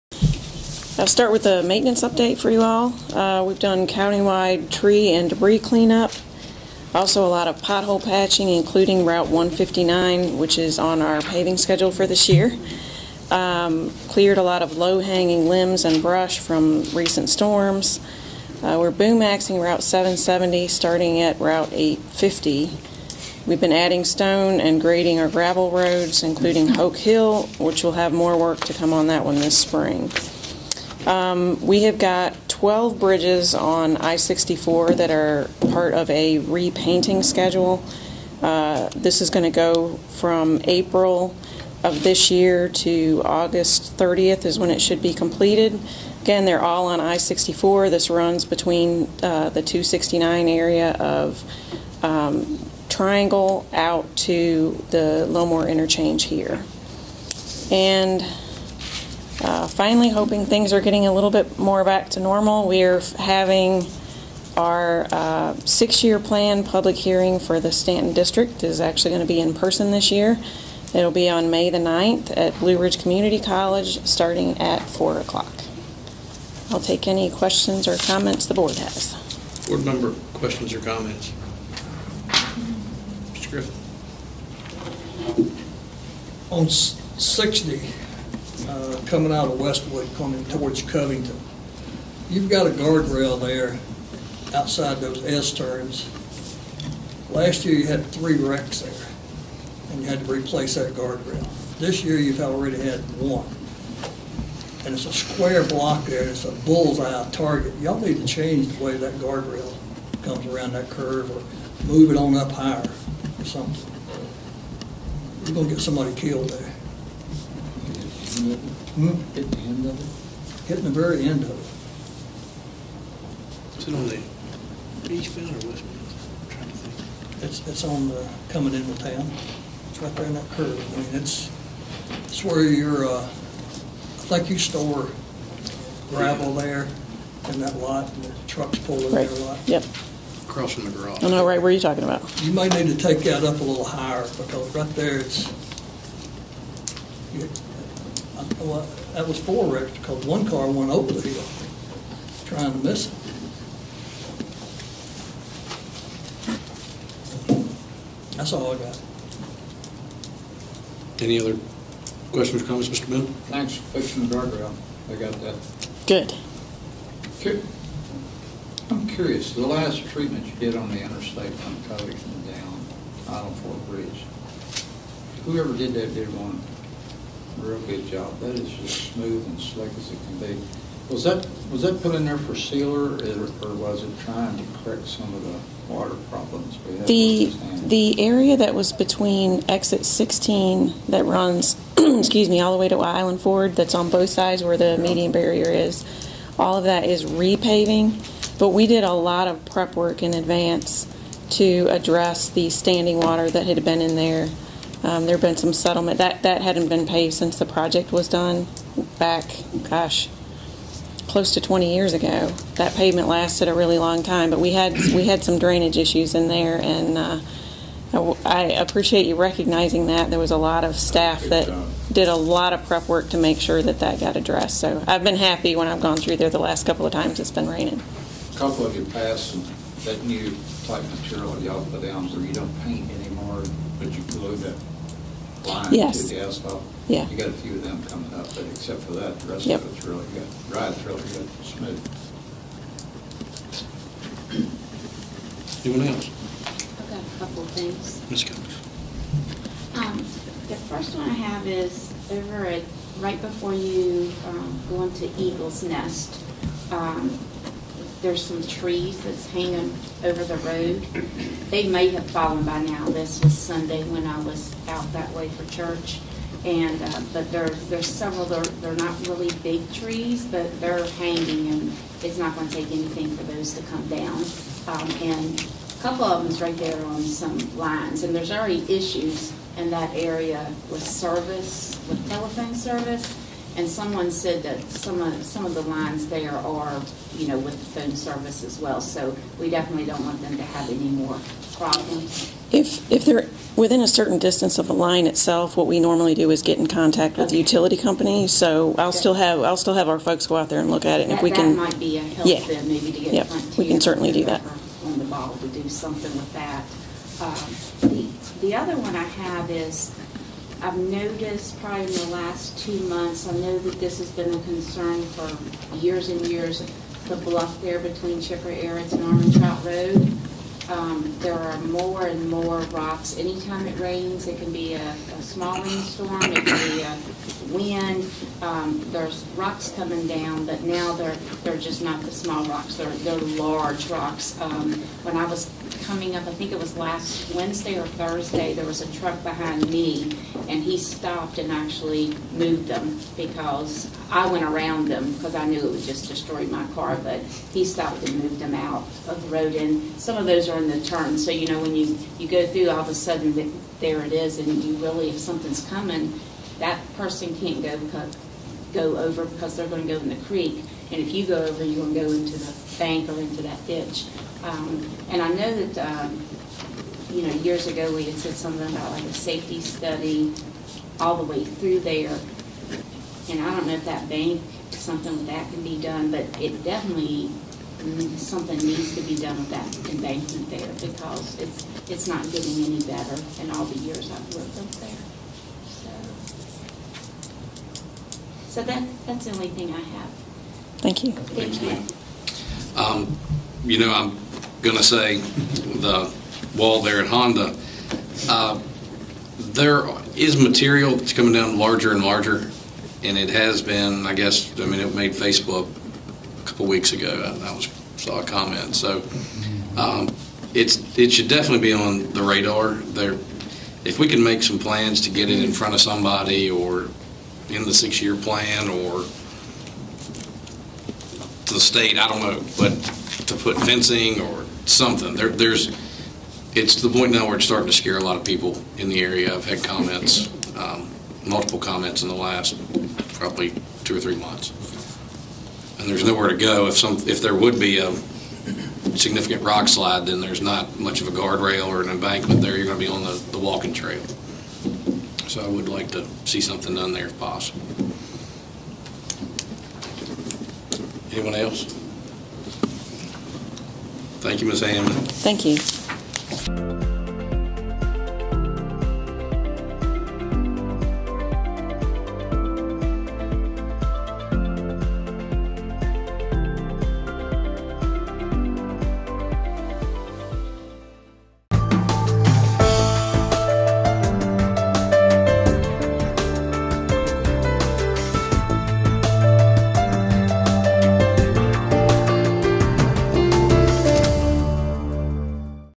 Hammond Updates Alleghany Co. Sups On Road Maintenance Issues
VDOT's Susan Hammond updated the Alleghany County Board of Supervisors on area road maintenance issues at the meeting held Tuesday, March 1, 2022.